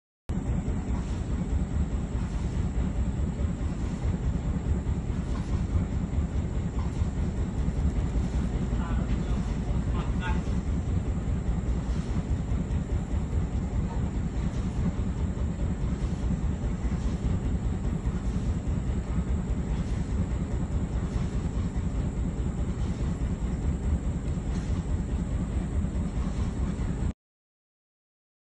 Floating Plants Blocking The River 🐊
This is at Tanjung Puting National Park.
normally rainy season the water level is very high and the plants around the river is broken their roots and after that they will go up and down the river depending the current of water in the river and very often it can block the river.